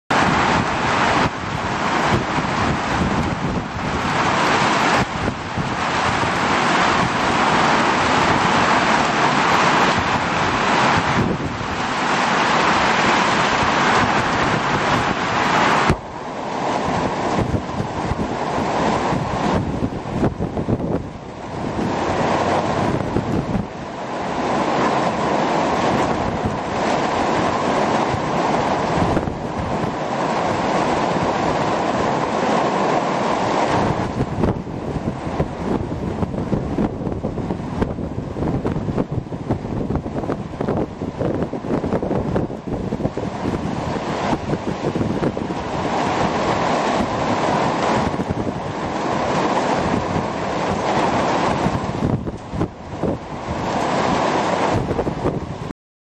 Video 2# - Torrential rain and strong winds
Video 2# - Torrential rain and strong winds yesterday in Hamilton